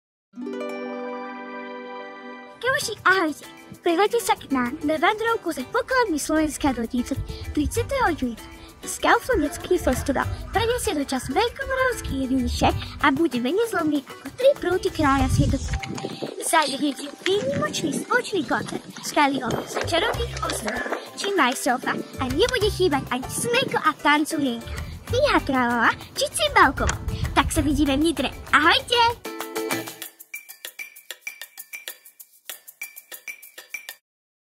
Kaufland detský festival - Na vandrovke za pokladmi Slovenska
TO BUDE SUPER LETO: KALI AJ  MAJSELF VYRÁŽAJÚ PO SLOVENSKU ZA POKLADMI, VIDÍME SA V NITRE?

Ako zažiť s deckami super leto a spoznávať pri tom krásy nášho Slovenska? Poďte s nami na vandrovku, ktorá vás spoločne s Kaufland Detským festivalom vezme za pokladmi našej krajiny, o akých ani učebnice nepíšu.
V nedeľu 30. júna sa pripravte na veľké dobrodružstvo v Nitre. Na festivalovú slávnosť v štýle Veľkomoravskej ríše sú pozvaní aj Kali, Majself, či Paci Pac. Prídete aj vy?

Po stopách Veľkej Moravy a jej pokladov

Zaujíma vás, ako sa žilo v časoch Veľkej Moravy, veľkolepej ríše, ktorú v  9. storočí založil knieža Rastislav, keď k svojej ríši pripojil Nitrianske kniežatstvo? V časoch, keď k nám Solúnski bratia Cyril a Metod priniesli hlaholiku, jedno z dvoch najstarších slovanských písem? Alebo vás láka čaro sokoliarskeho umenia, ktorému sa kedysi dávno venoval aj Svätopluk, legendárny panovník so slávou i mocou kráľa?

Ak ste na niektorú z týchto otázok odpovedali kladne, bola by veľká škoda, ak by ste v poslednú júnovú nedeľu na Kaufland Detskom festivale obišli Veľkomoravské mestečko K-Z lásky k tradícii. Na vlastné oči tu uvidíte rytierske brnenie, aké nosil knieža Pribina, priučíte sa stredovekým remeslám kováčov či košikárov, či čaru hlaholiky na kurze kaligrafie, alebo si zmeriate sily pri lámaní legendárnych troch prútov Svätopluka.

Pre maškrtné jazýčky je pripravená ochutnávka produktov K – Z lásky k tradícii, ktorú pre Kaufland vyrábajú najlepší slovenskí dodávatelia. Ak sa so svojimi ratolesťami zapojíte do rodinnej súťaže v miesení cesta na medovník, váš deň bude ešte sladší. Nitriansky región je totiž celé storočia známy umením pernikárov a medovnikárov.

Nebudú chýbať obľúbení moderátori Martin Madej a Veronika Hatala, unikátny projekt Cimbalkovo, ktorý spojil mladé talenty z celého Slovenska, Majself, či  vystúpenie Kaliho a Petra Panna so  súborom Čarovné ostrohy.
Deti si užijú svoj deň spoločne so Smejkom a Tanculienkou, škriatkom Fíha Tralala, či Cupi &Lupi.

Letnú vandrovku „Po stopách pokladov“ vám nielen v Nitre, ale aj v ďalších slovenských mestách aj tento rok zadarmo prináša Kaufland. Pretože naša domovina je plná vzácnych pokladov, ktoré stoja za to, aby ste ich objavili. Z lásky k Slovensku.

Tešíme sa na vás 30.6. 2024 v Nitre.

Ak to do Nitry máte ďaleko, program môžete sledovať nielen na káblovej rodinnej Turbo TV, ale aj online na